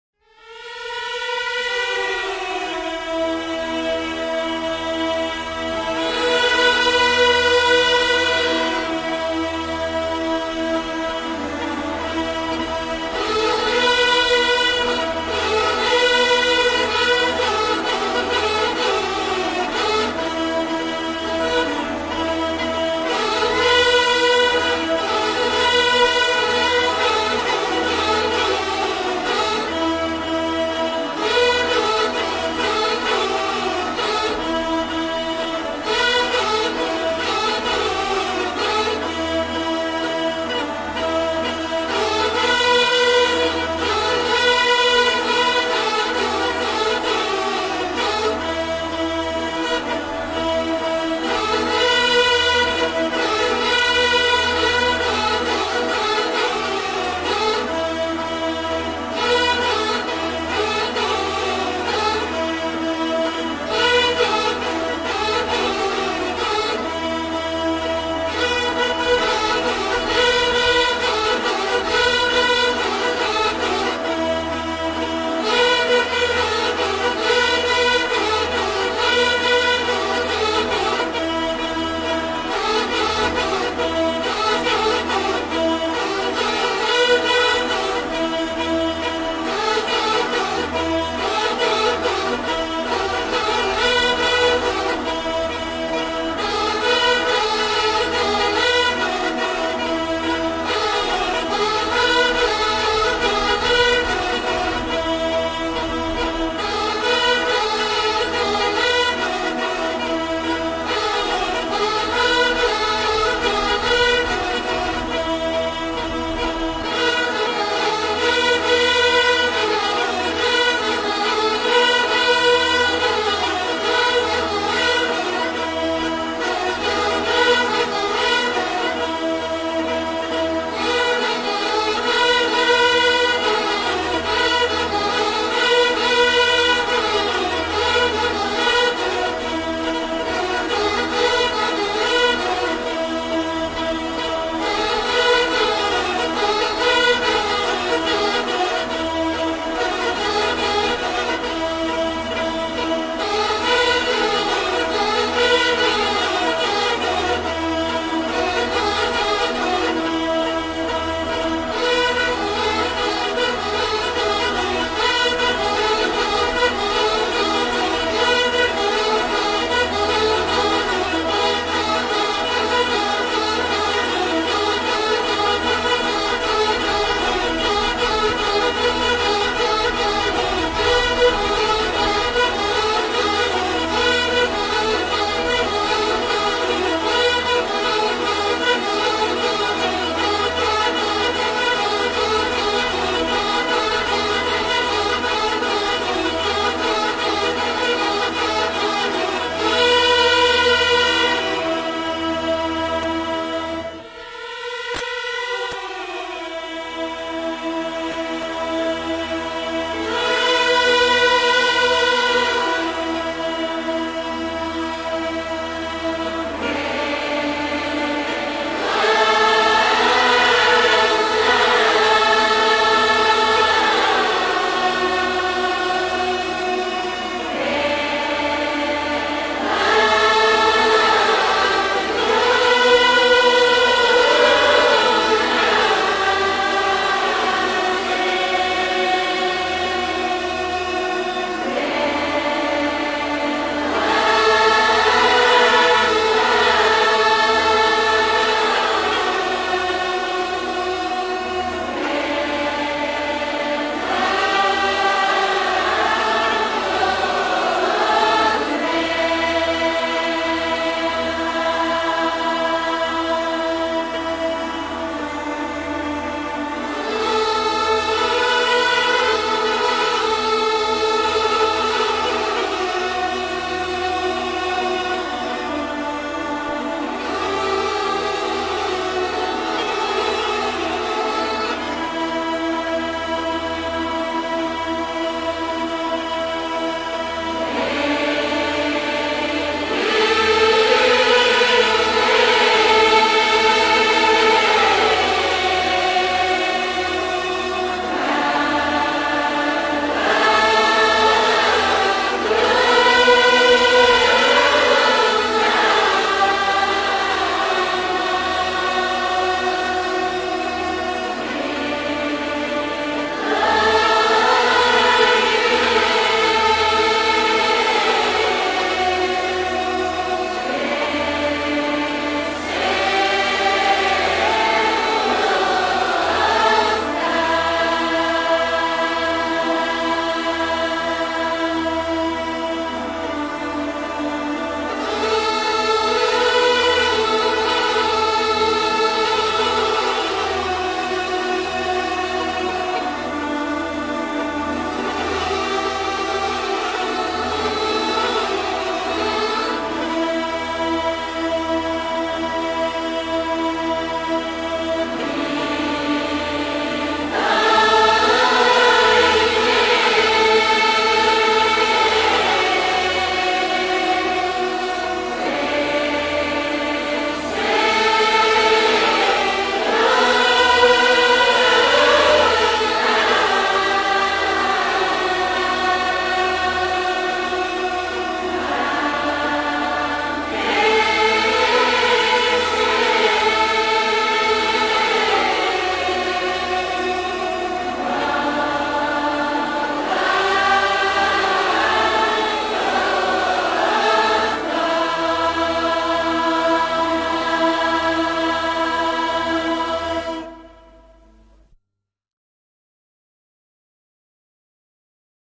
Bulgaria 100 kaba gaidi 3
Tova e edno video koeto sam napravih kato podbtah razlichni gledni i zabelejitelnosti ot predavaniq za BG kakto i izpolzvam narodna muzika na 100 kaba gaidi RESHIH DA NAPRAVIA TOVA VIDEO ZASHTOTO NIAMA V  NETA TAKIVA S HD KACHESTVO